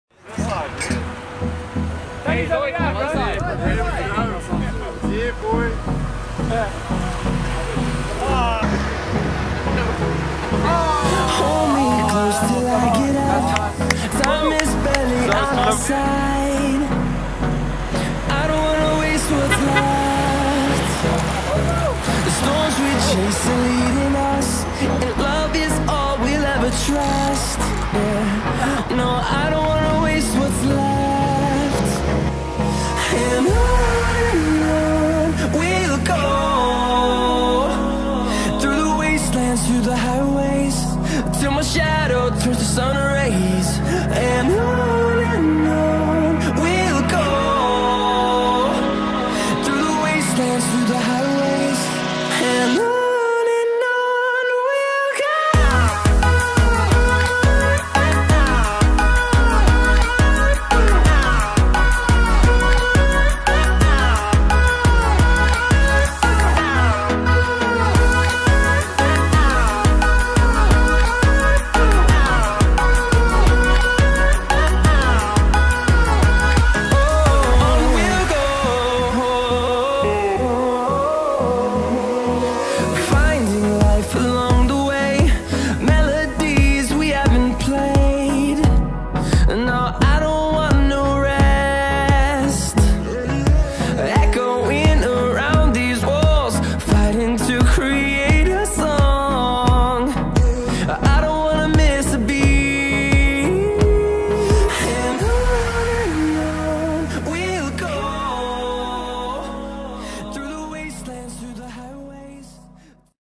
Lloyd's Bachelors Party
Our weekend spent in Rustenburg celebrating the soon to be husband.